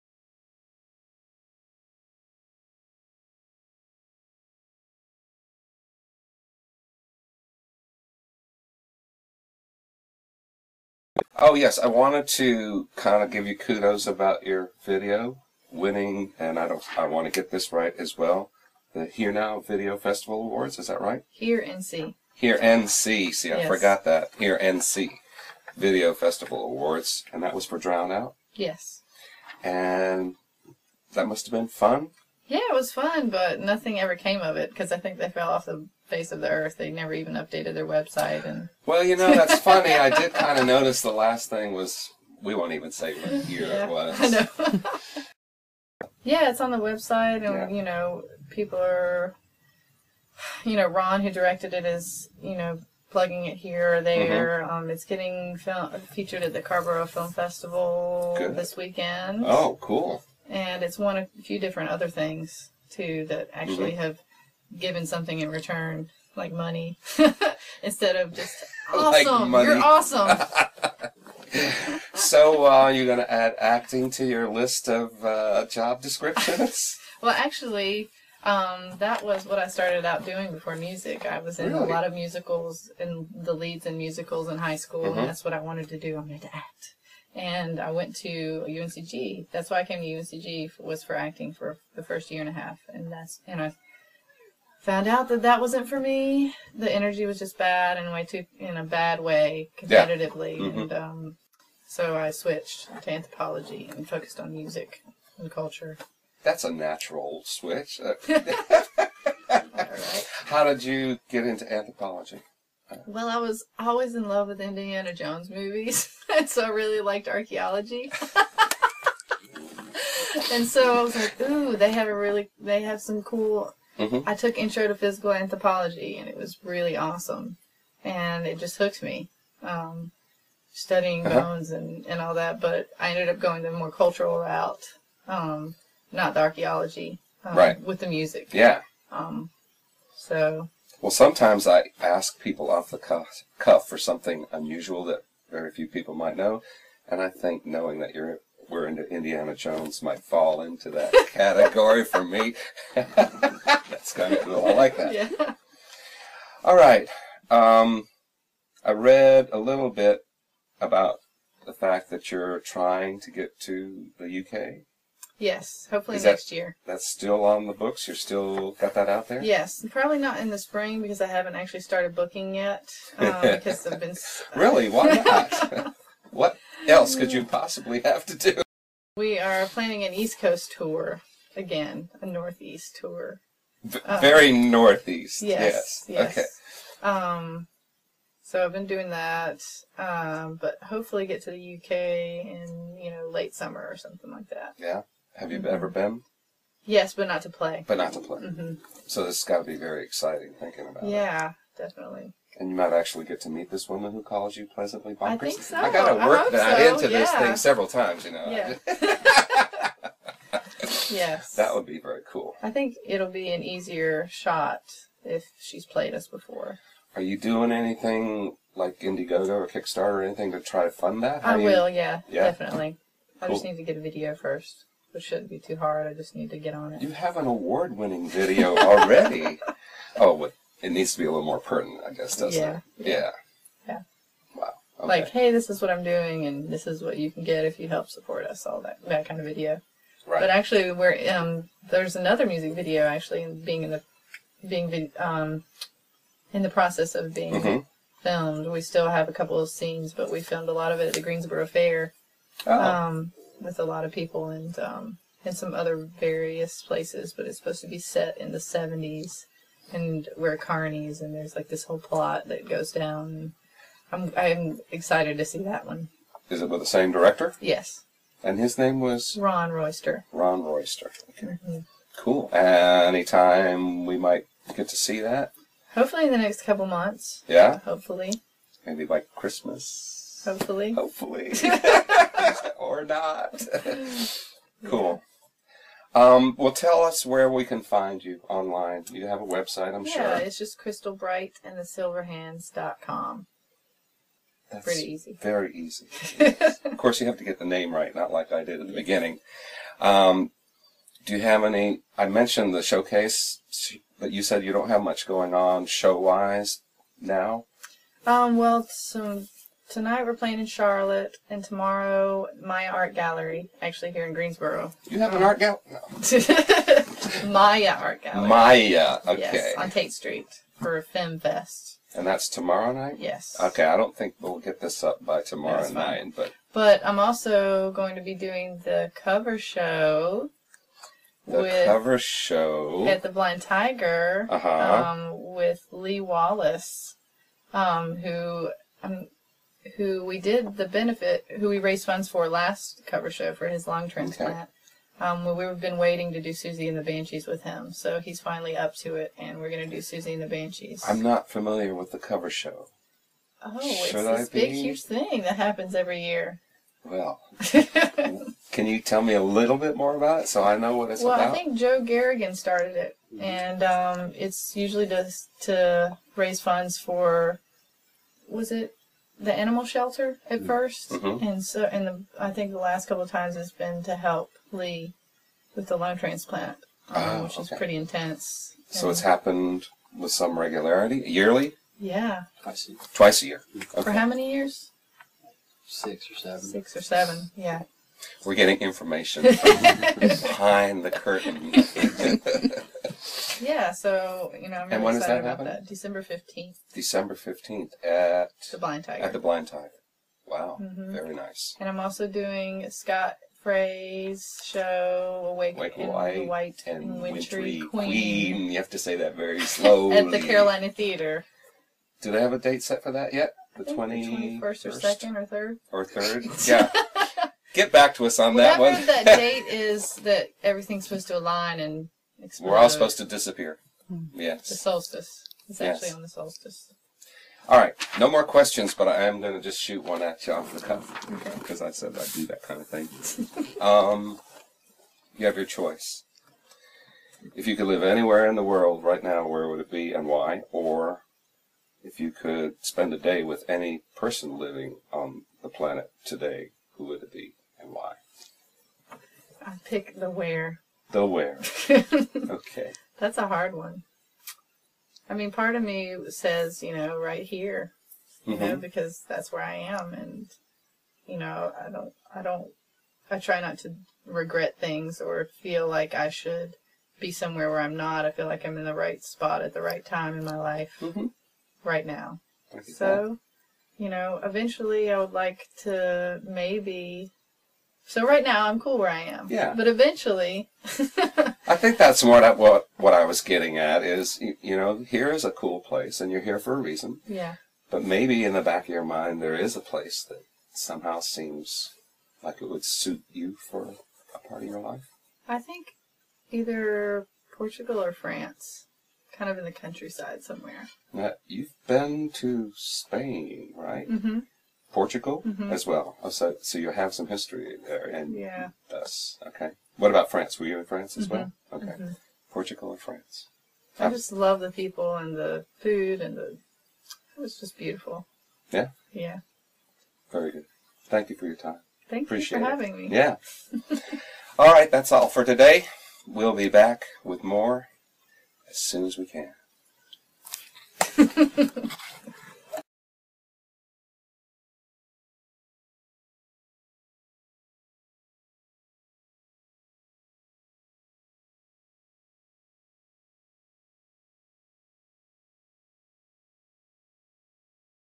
Kris Ferris Featured Artist interview with Crystal Bright, Part 3
As we get into the final segment of the interview with Crystal, we begin by talking about a video the band received an award for at the Hear NC Video Festival. From there we learn how acting started out being an early interest for Crystal, but when that became less desirable a love of the "Indiana Jones" character and movies led her down a whole new learning path. Then it's on to finding out about some recent gigs as well as what's in store for the band over the coming week and months.